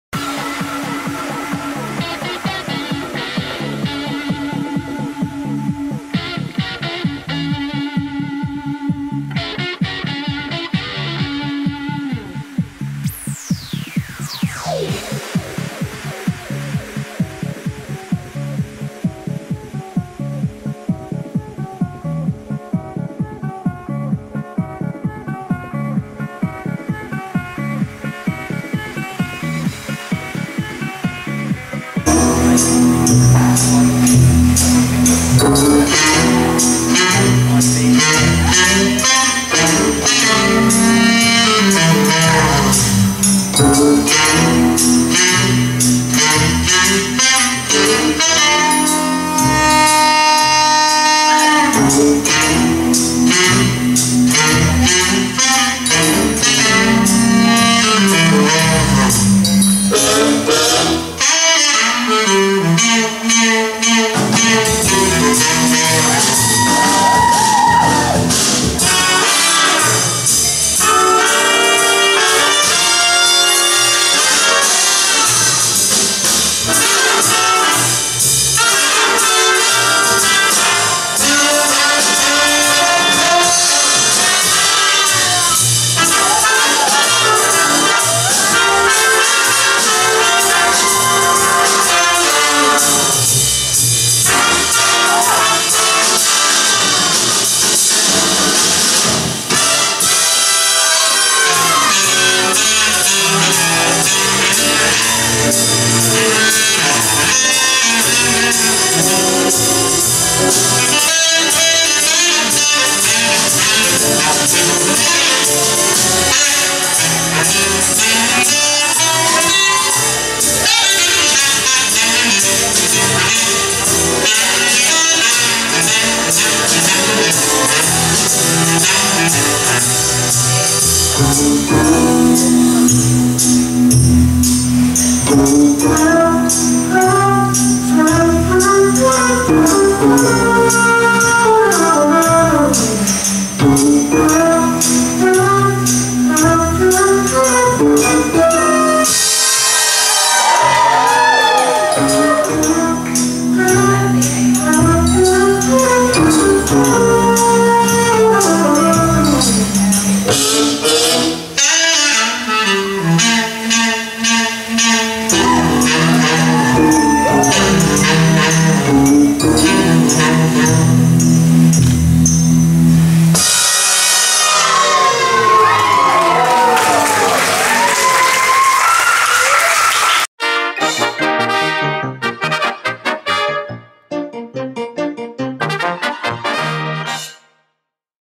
Cabaresque for Cancer 2016 Moreorlesque
The gorgeous Bethany Blue from Moreorlseque performing for Cabaresque for Cancer 2016